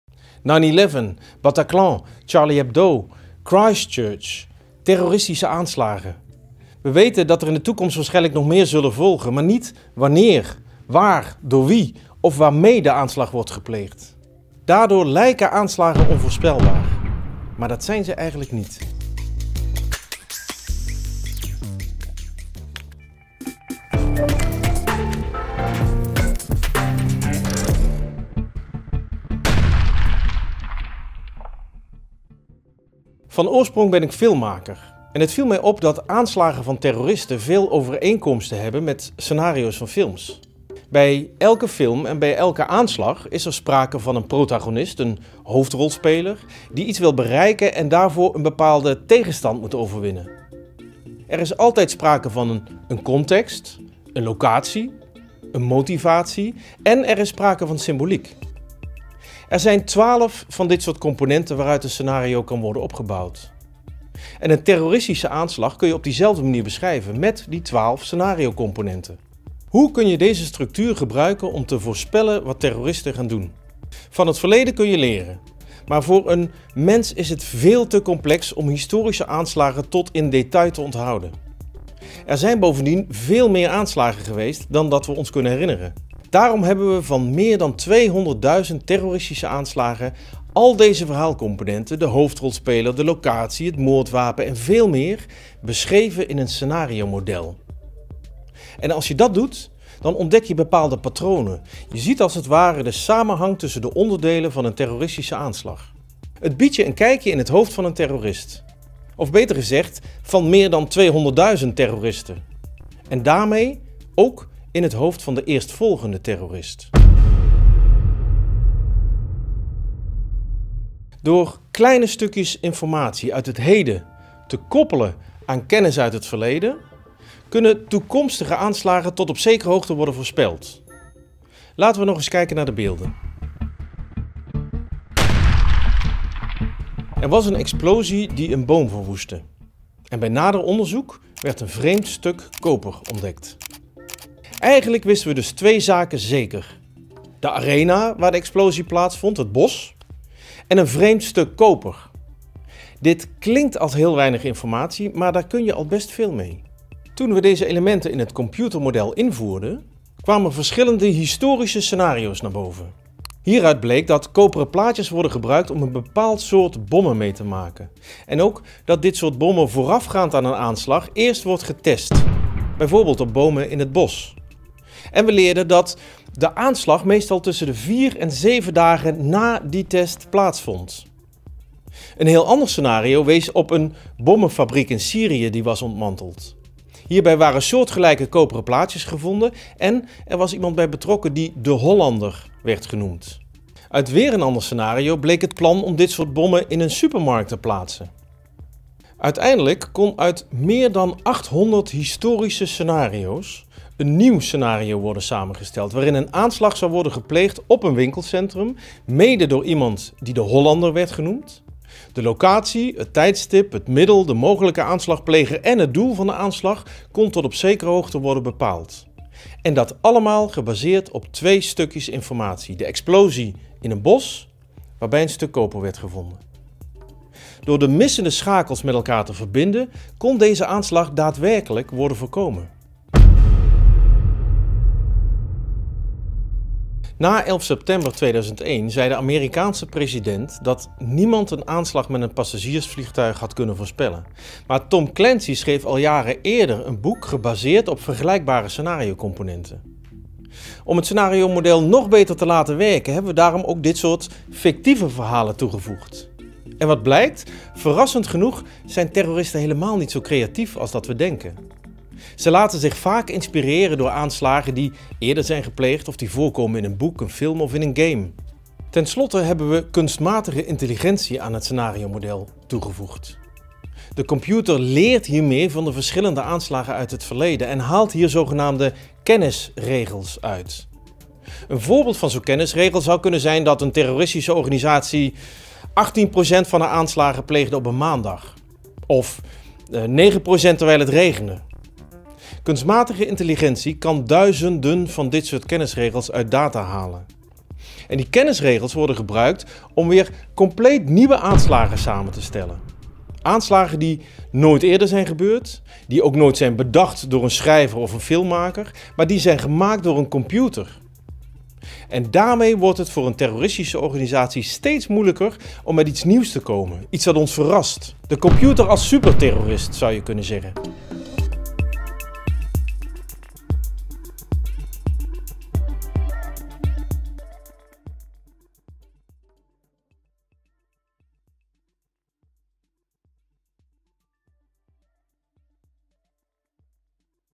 Hoe kunnen we ons beter voorbereiden op aanslagen?
20 jaar na #9/11 lijken aanslagen ons nog steeds te overvallen. Opeens zijn ze er, alsof niemand ze had zien aankomen. Maar is dat wel zo? Terrorismedeskundige Peter de Kock (Tilburg University) ontwikkelde een model waarmee we aanslagen steeds beter in kaart kunnen brengen en toekomstige aanslagen mogelijk zelfs kunnen worden voorkomen.

Meer weten over het onderzoek van Peter de Kock?
Je vindt zijn studie 'Anticipating criminal behaviour: Using the narrative in crime-related data'